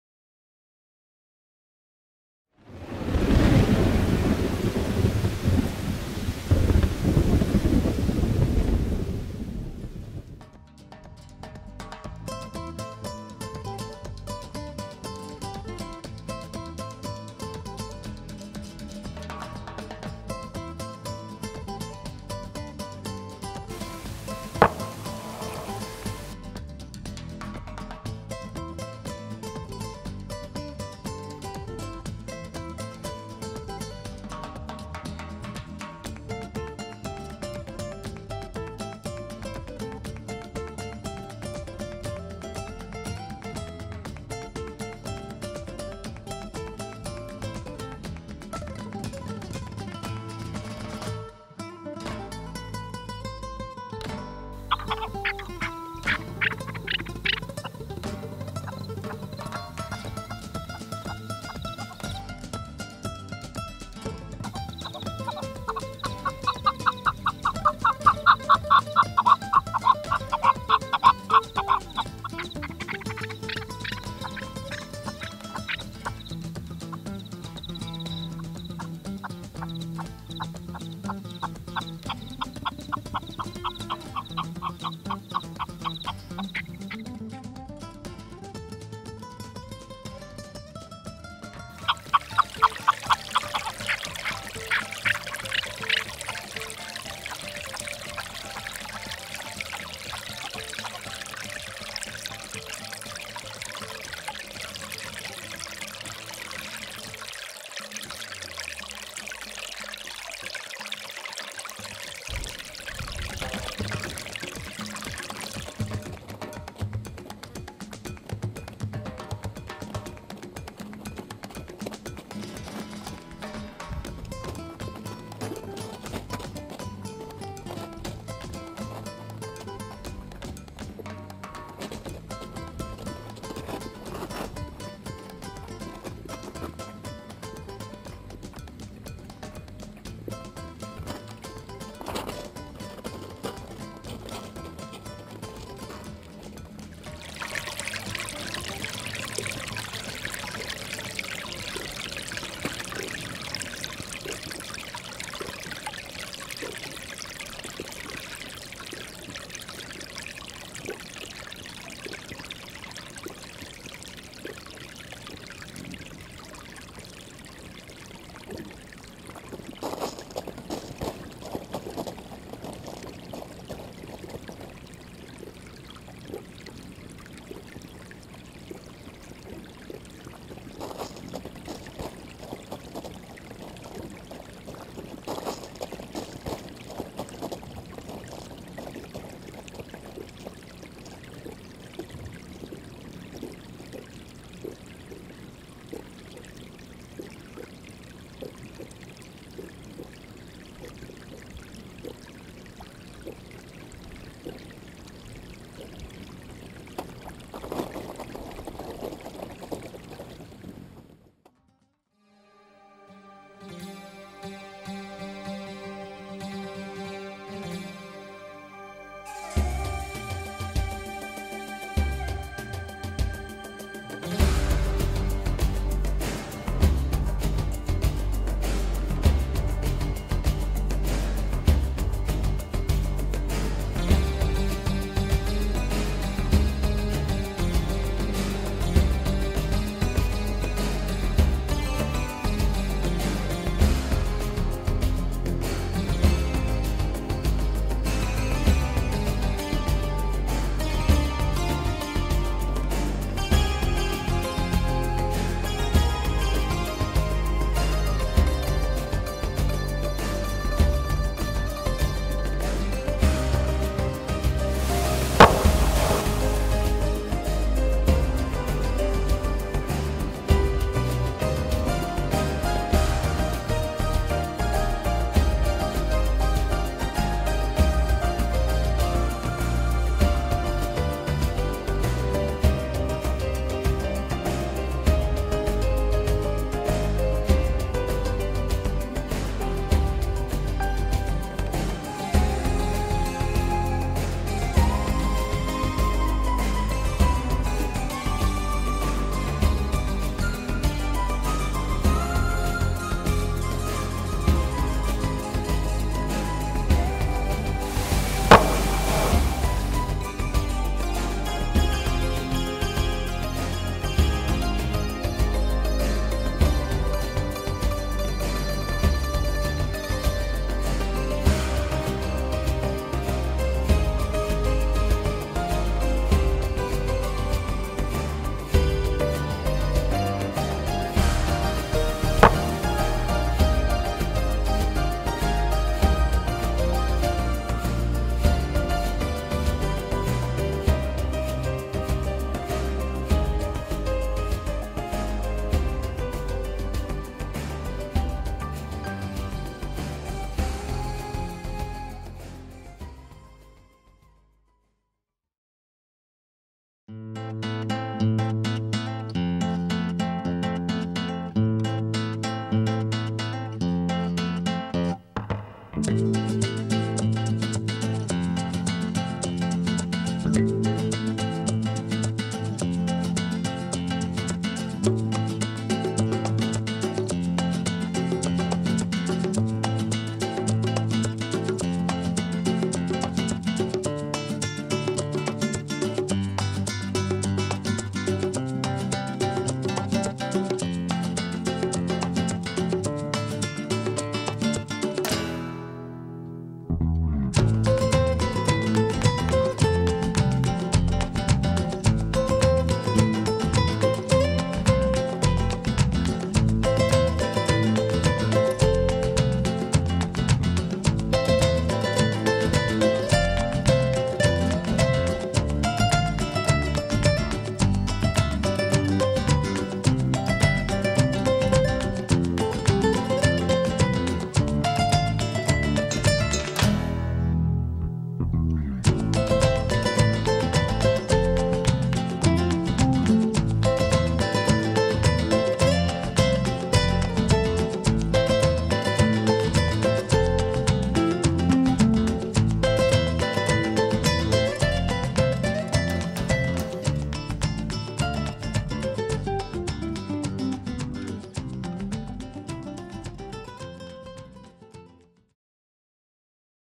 Desert bighorn sheep 2015
This 7 1/2 minute video highlights the desert bighorn sheep in Nevada. From the big rams to the next generation I'm sure you will enjoy. Hopefully this will be playing at the Nevada Department of Wildlife booth during the 2015 annual sheep show in Reno.